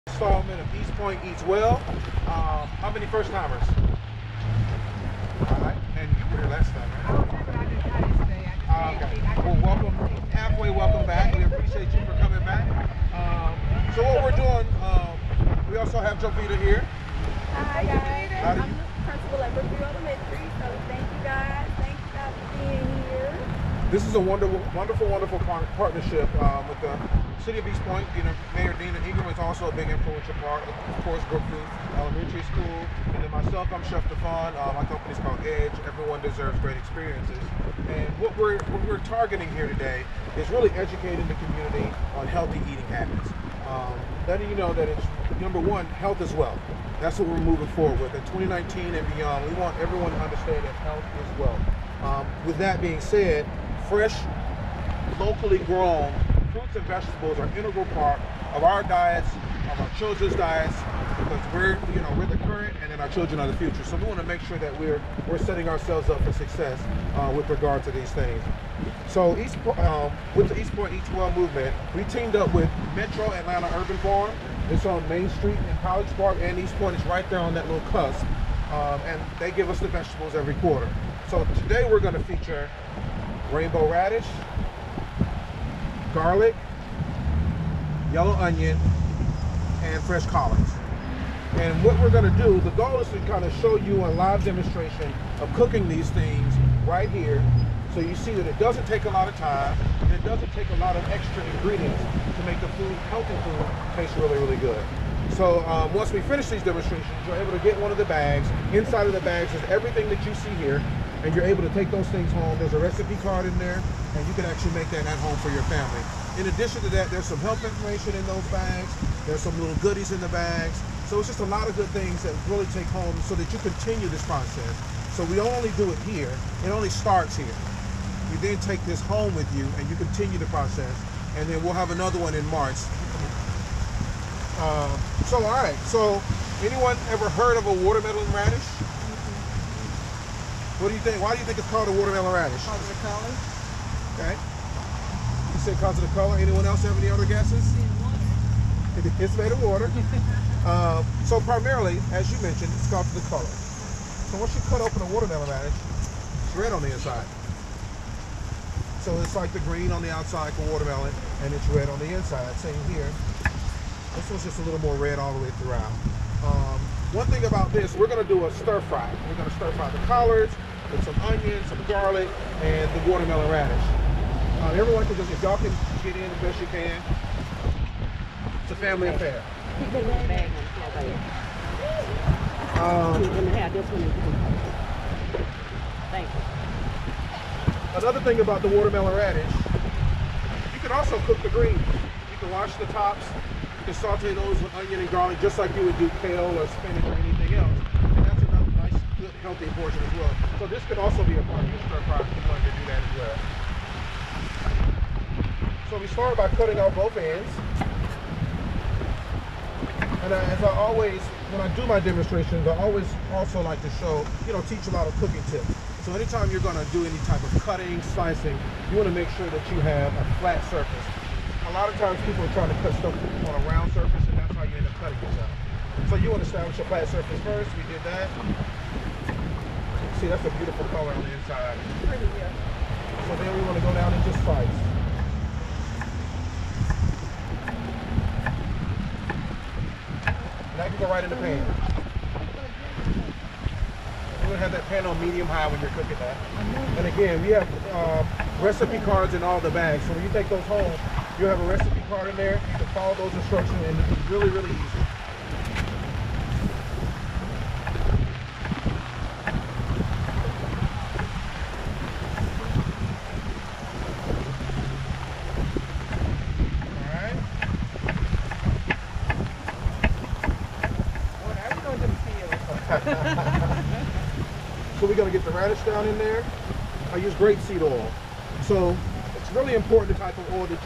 DEMO: Chef Dephon - East Point Eats Well
Chef Dephon cooking a Collard Greens and Watermelon Radish dish teaching the community about healthy eating